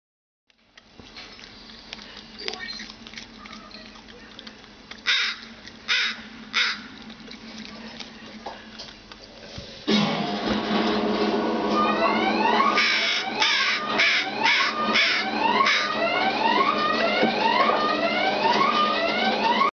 Uninvited Dinner Guest
Racoon eats Pretty Boy's food.  Pretty Boy won't come in the house due to my cats' aggression toward him so now he's living on my front porch.  Any leftovers are devoured by raccoons, possums or other cats.